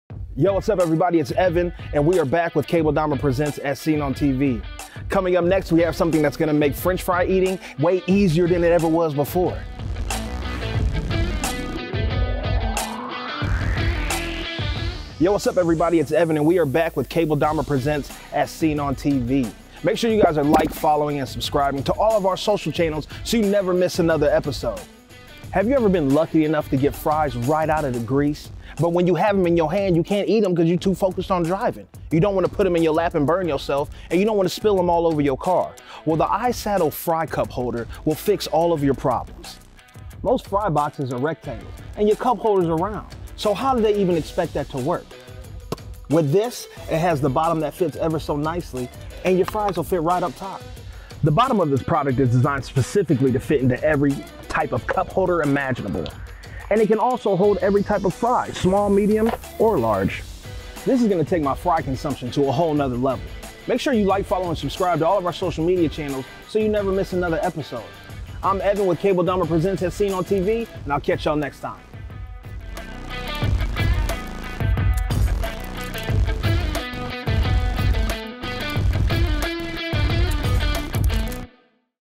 FRENCH FRY CUP HOLDER FOR YOUR CAR!!! | CABLE DAHMER
In this week's episode of Cable Dahmer Presents: As Seen On TV, Evan reviews the iSaddle French Fry Cup Holder!

If you want to pick this up for yourself, here's the link!